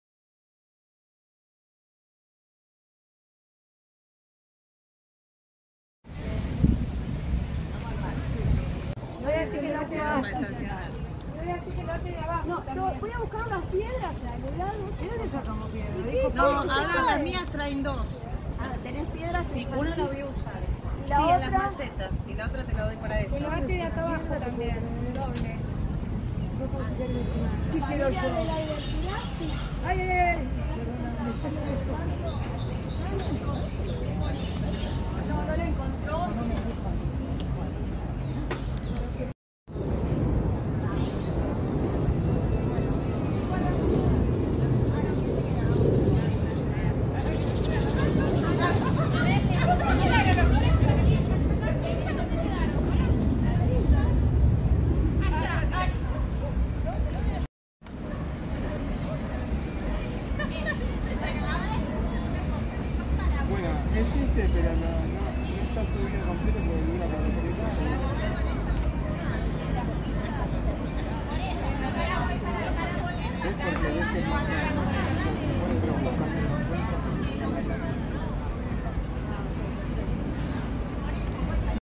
Feria ambiental 2024
El turno tarde de los jardines de infantes tuvo el mismo inconveniente que el turno mañana: se cierran los afiches hasta último momento y el personal de mantenimiento del parque no deja piedras en su interior para que no se tiren entre sí. Los preparativos tuvieron como diferencia un gazebo.